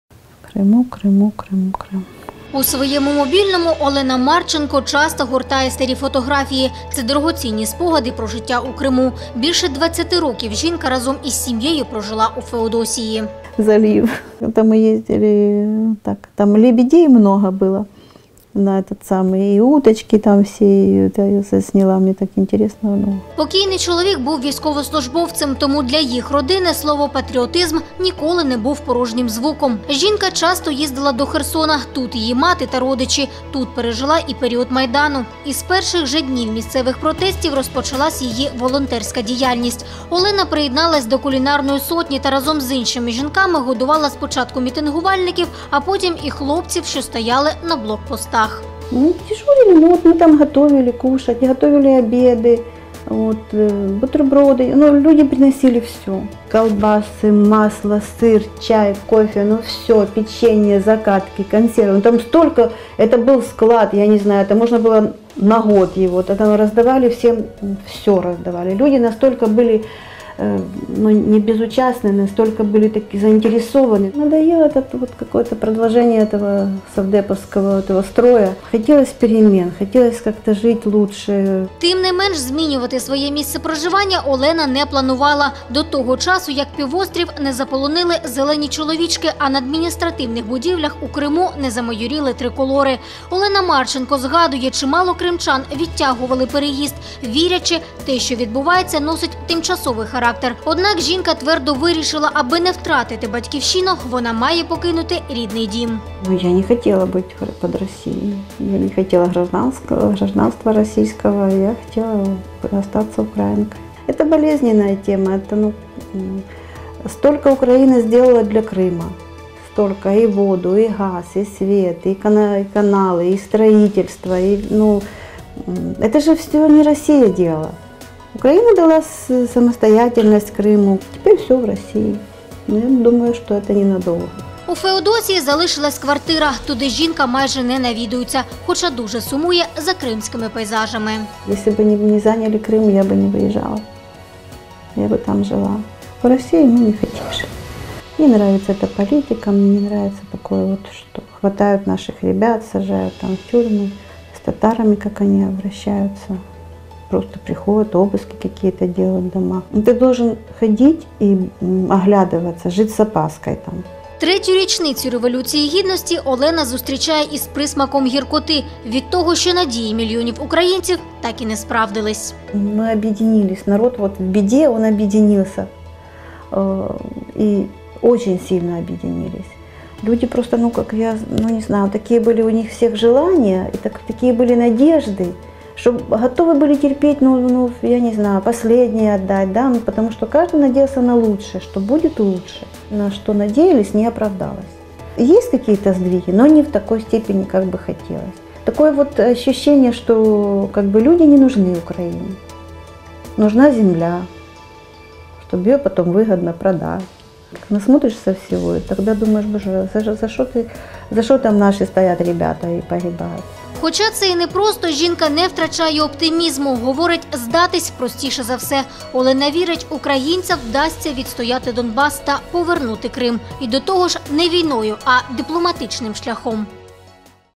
Олена Марченко: "Аби не втратити Батьківщину, я покинула рідний дім"
У своєму мобільному Олена Марченко часто гортає старі фотографії. Це дорогоцінні спогади про життя у Криму. Більше 20 років жінка разом із сім’єю прожила у Феодосії. Покійний чоловік був військовослужбовцем. Тому для їх родини слово «патріотизм» ніколи не було порожнім звуком. Жінка часто їздила до Херсона, тут - її мати та родичі. Тут пережила і період Майдану. Із перших же днів місцевих протестів розпочалась її волонтерська діяльність. Олена приєдналась до кулінарної сотні, та разом з іншими жінками годувала спочатку мітингувальників, а потім і хлопців, що стояли на блокпостах.